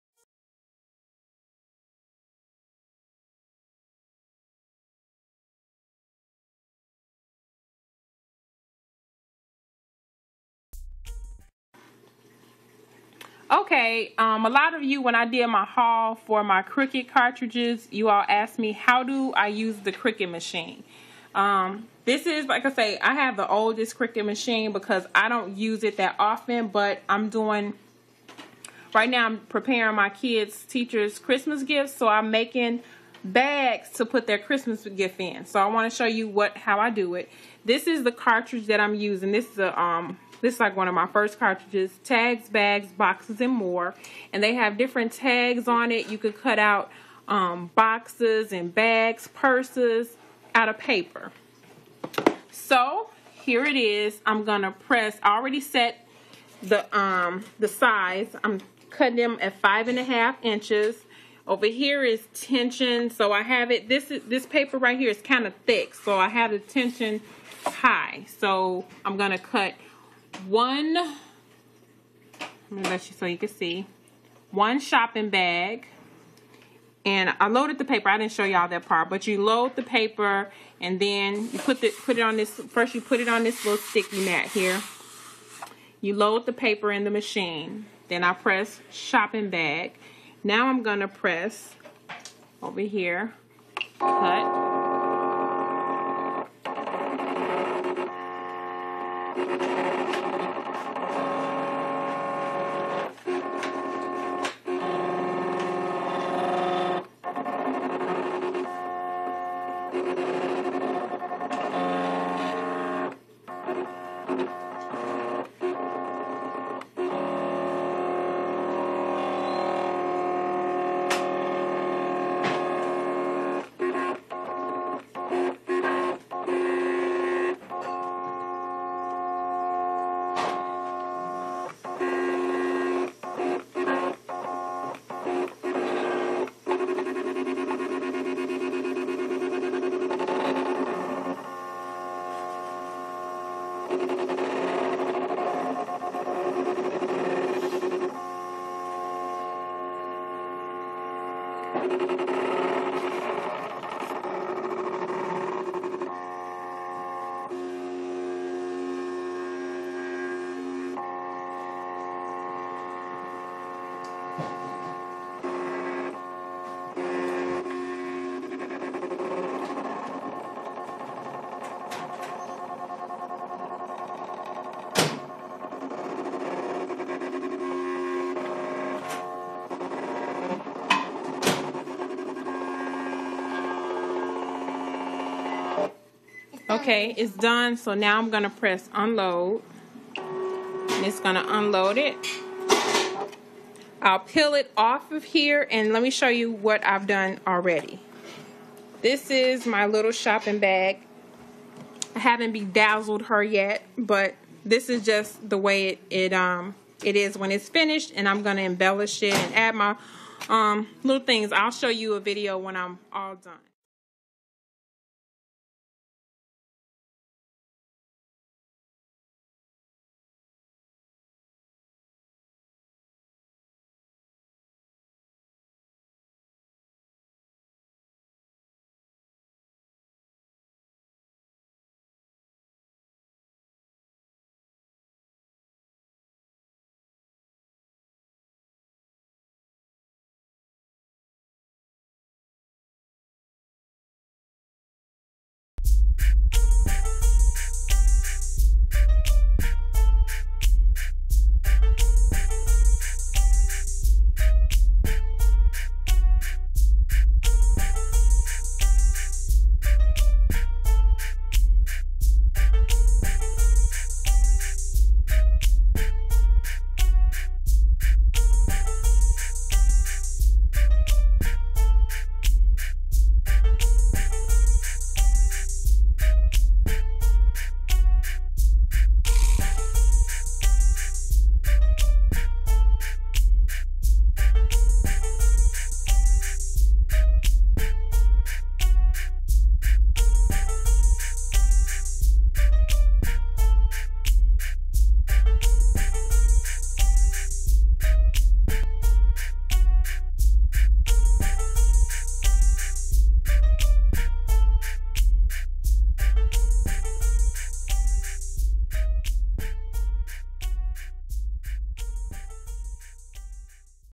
Copy of How I Use My Cricut Machine
When I did my black friday haul, many of you asked how does the cricut machine work.  I'm making Christmas gifts for Teachers and Coaches so, here's a tidbit.